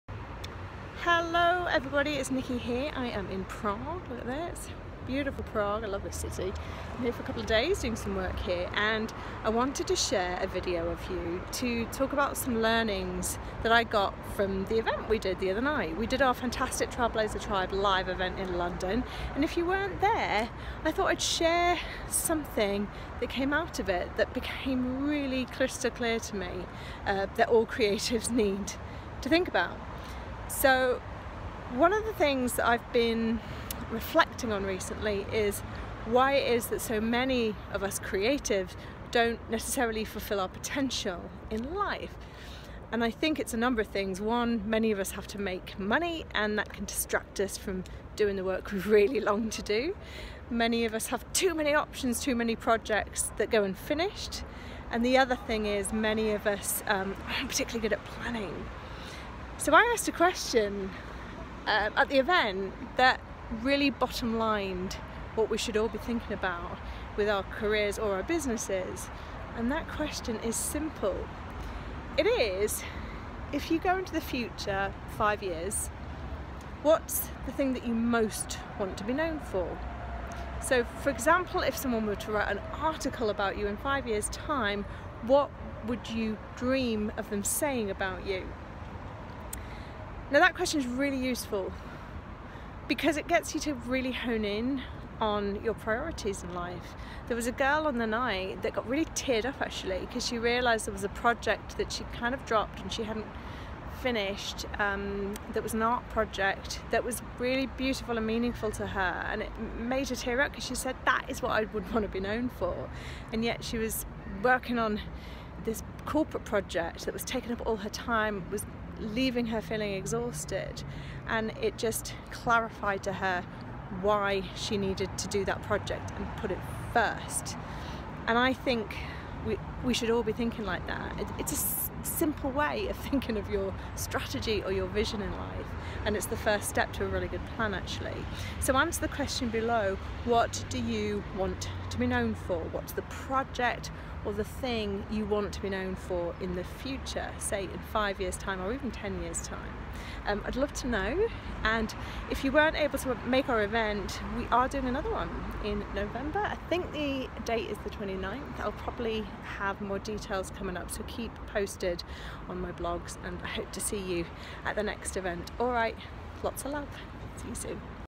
The one question all creatives should ask themselves
For all creatives, this video teaches the key question to think about for planning a great career. Creative careers are not usually linear - they can be unconventional and multi faceted. However, without asking good questions, it's possible to go in circles. Check